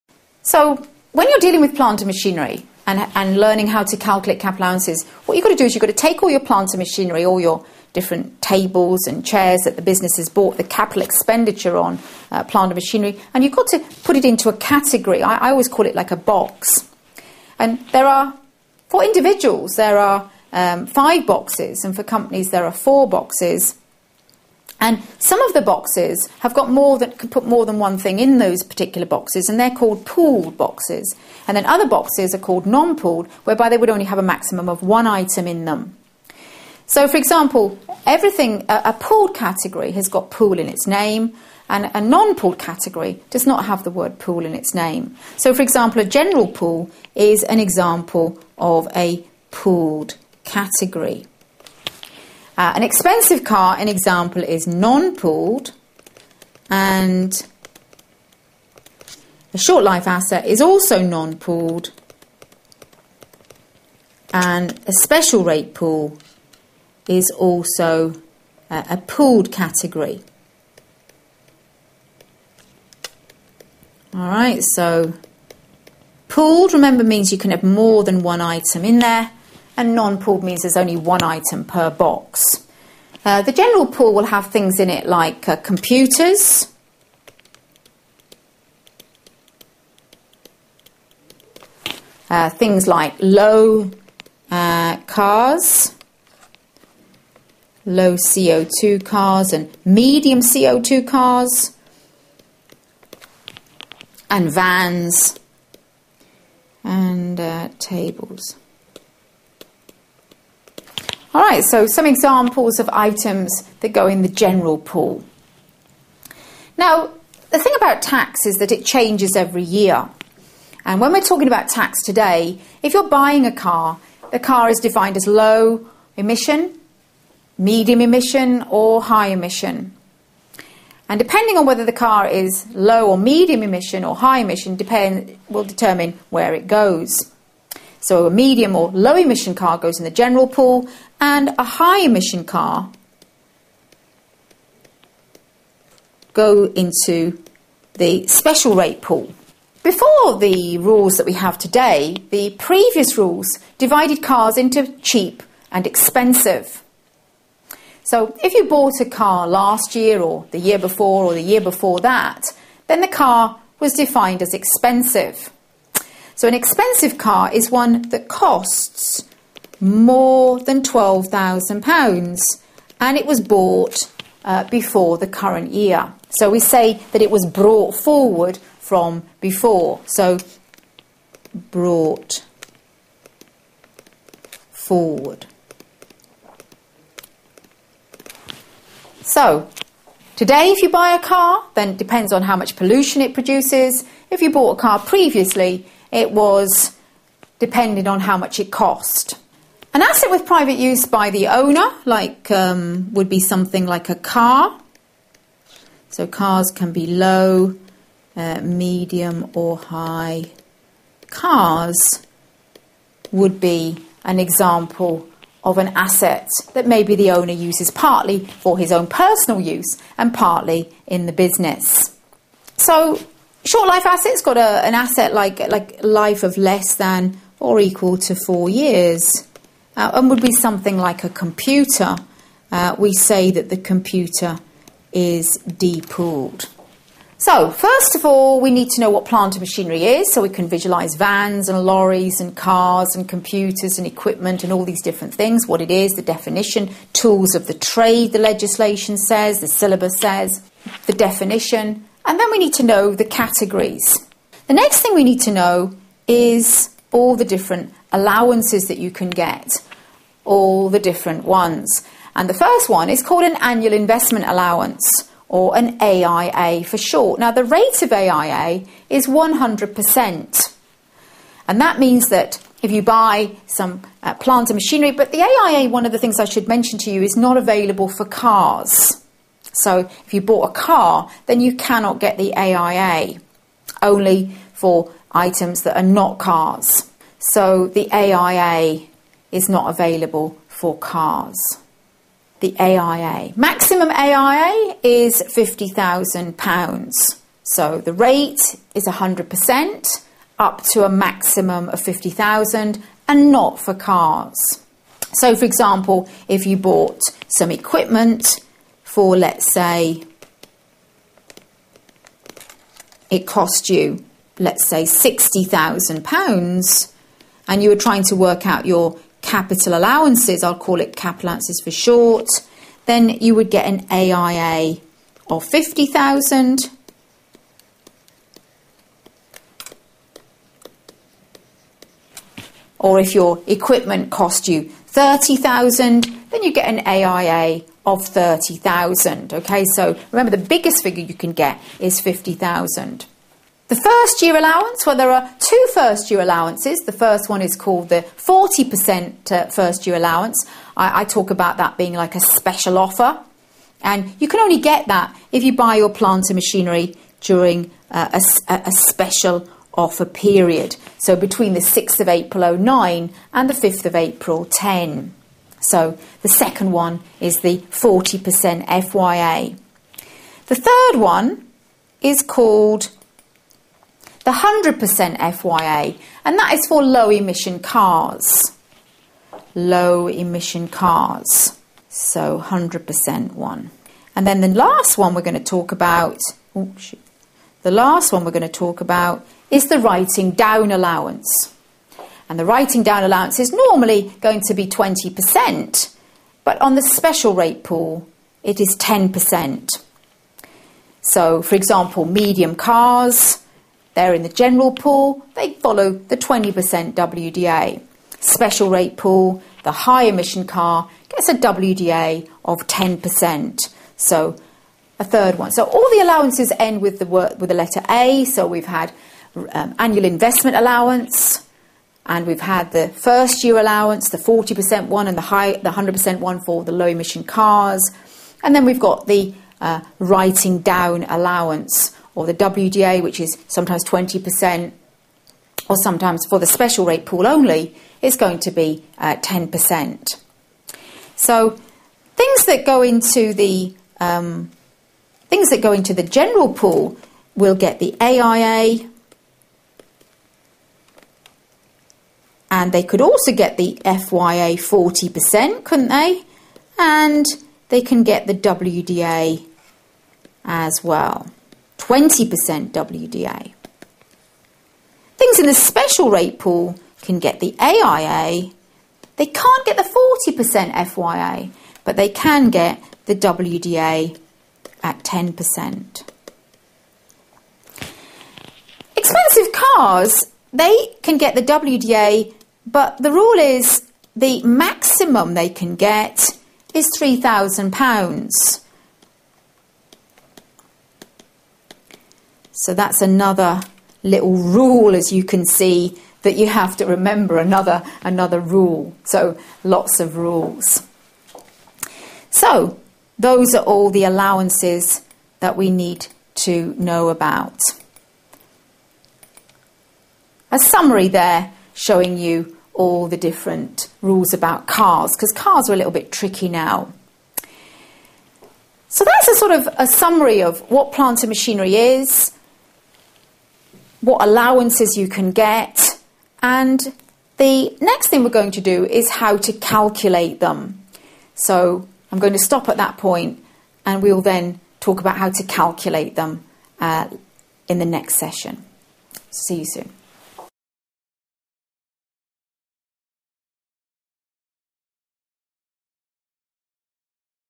LSBF ACCA F6: Introduction to Capital Allowance Part 2
London School of Business and Finance (LSBF)

Carolyn Napier presents an introduction to 'Capital Allowance', from the ACCA F6 exam. Watch now for an overview of this topic.